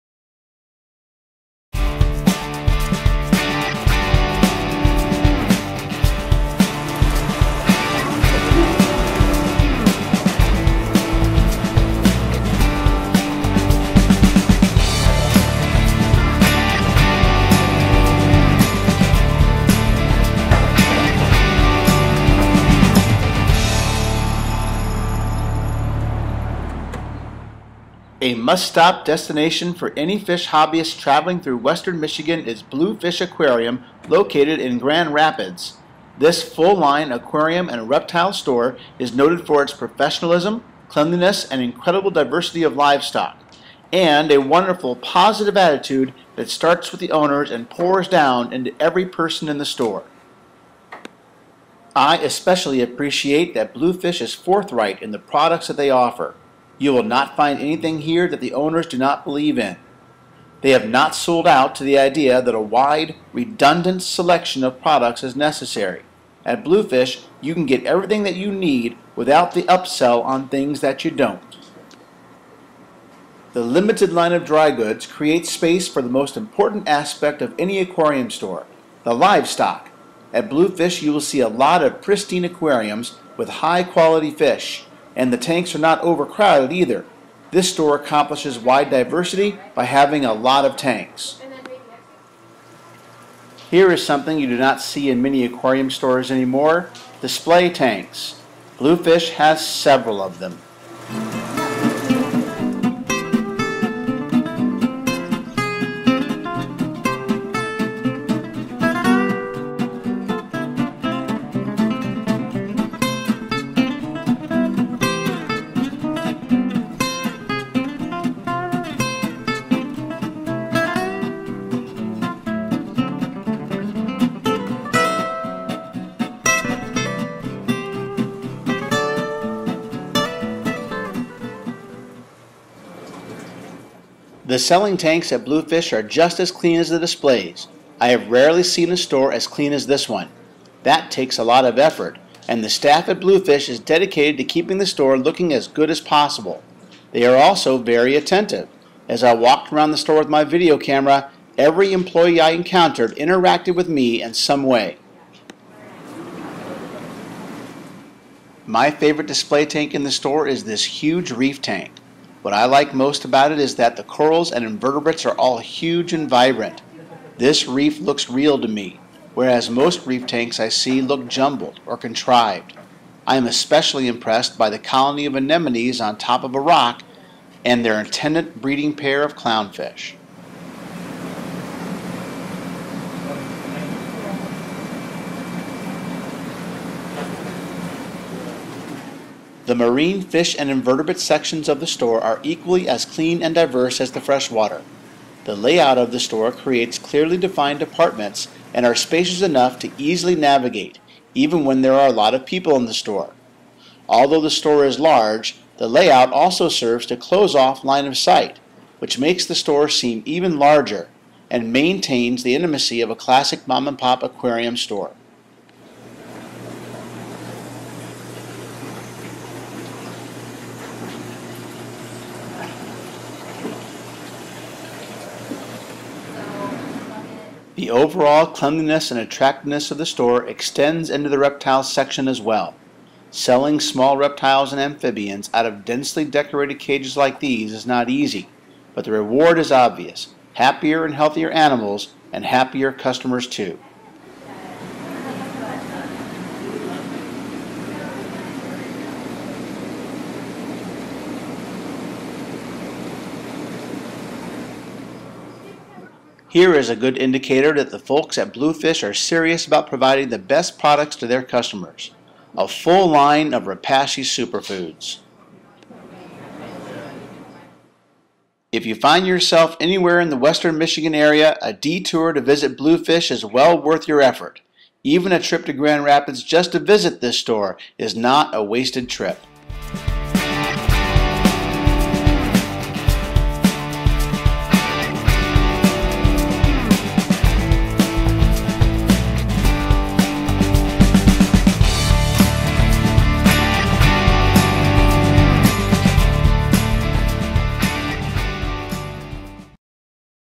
Blue Fish Aquarium
A tour of Blue Fish Aquarium, and excellent aquarium store in Grand Rapids, Michigan.